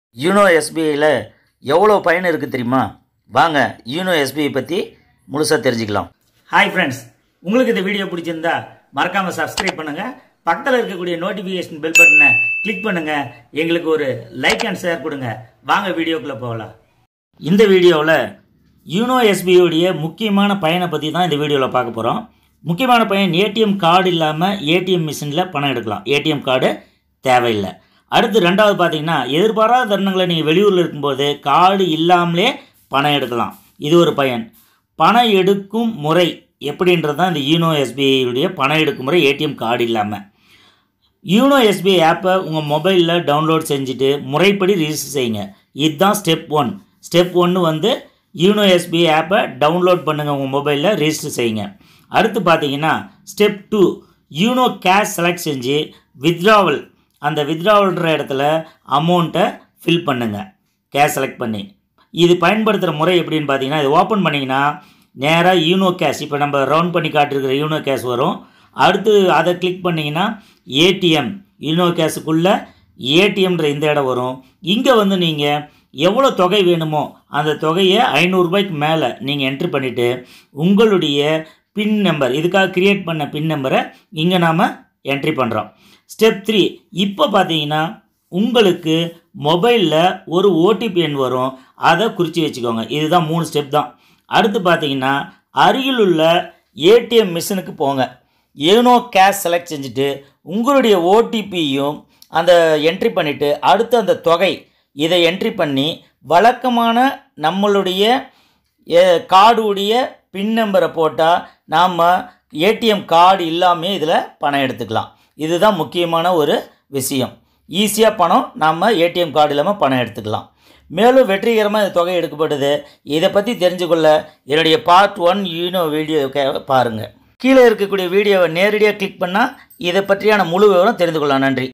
YONOCASH |CARDLESS| WITHOUT ATM USE செய்வதுஎப்படி? |tamil|learn to win tamil
This video explain how to use the yonosbi. Yonosbi is very useful and user friendly. Any place any atm corner no need to use atm card.  Yonosbi app  help to the customer to cordless transaction.  Yonosbi very very useful for all. Yono is very advanced app to introduced the state bank of india.
MY OTHER VIDEOS LINKS WATCH & SHARE & LIKE